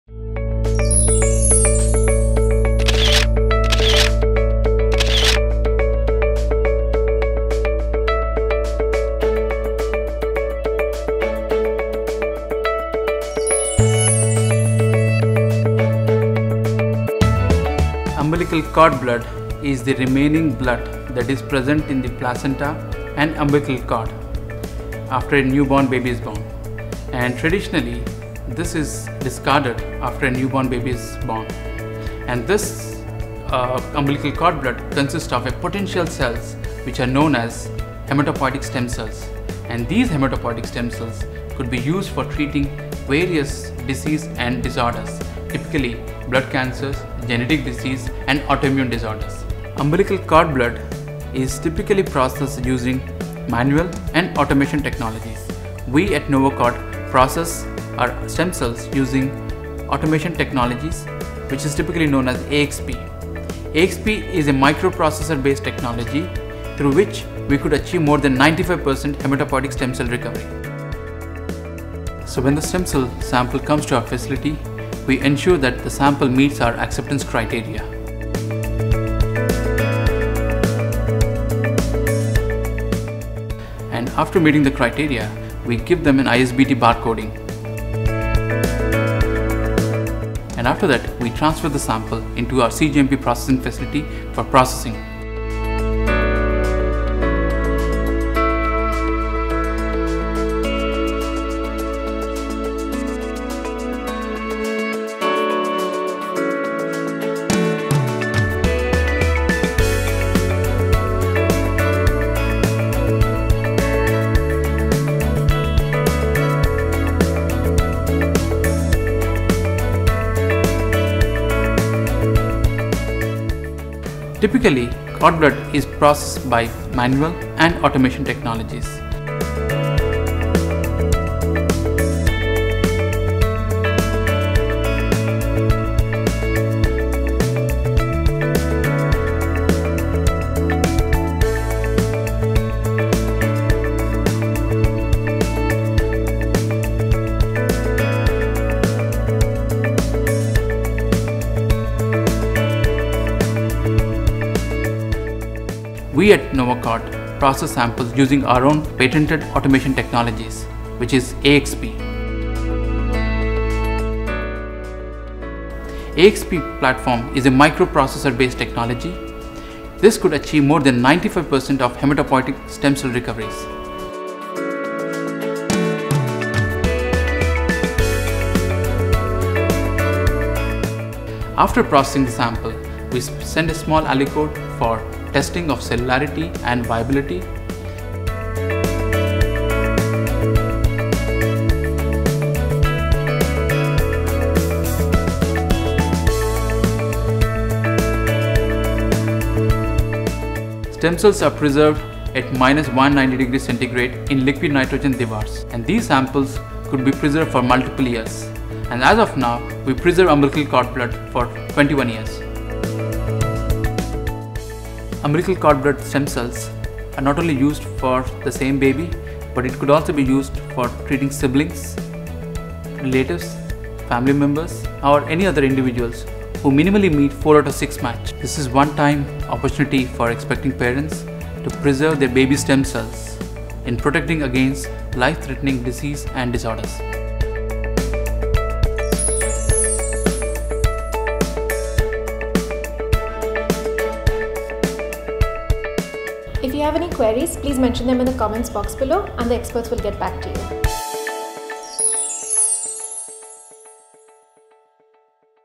Mamma's Journey: Episode 9
Swati visits the Novacord lab, India's 1st clinical cord blood bank, to know more about the technology and how cord cell banking can help to keep her baby safe against various diseases and disorders. Go behind the scenes on ‪#‎MammasJourney‬ to see how Novacord is breaching the final frontier of medical science.